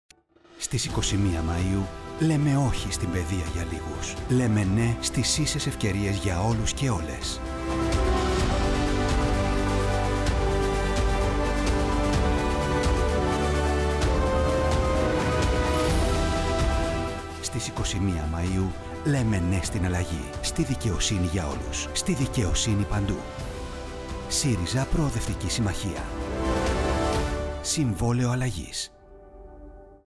Στις 21 Μαΐου λέμε όχι στην παιδεία για λίγους, λέμε ναι στις ίσες ευκαιρίες για όλους και όλες.
Στις 21 Μαΐου λέμε όχι στην παιδεία για λίγους, λέμε ναι στις ίσες ευκαιρίες για όλους και όλες. Ψηφίζουμε ΣΥΡΙΖΑ ΠΣ